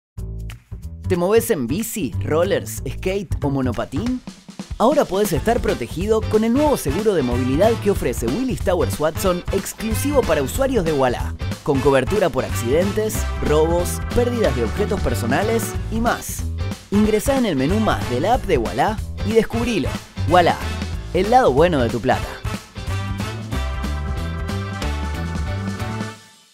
Nuevo seguro de movilidad | Ualá seguros y asistencias
¡Presentamos el nuevo seguro de movilidad que ofrece Willis Towers Watson, exclusivo para usuari@s de Ualá! Ya podés pagar el seguro para tu bici/ bici eléctrica, monopatín, rollers, skate y mucho más.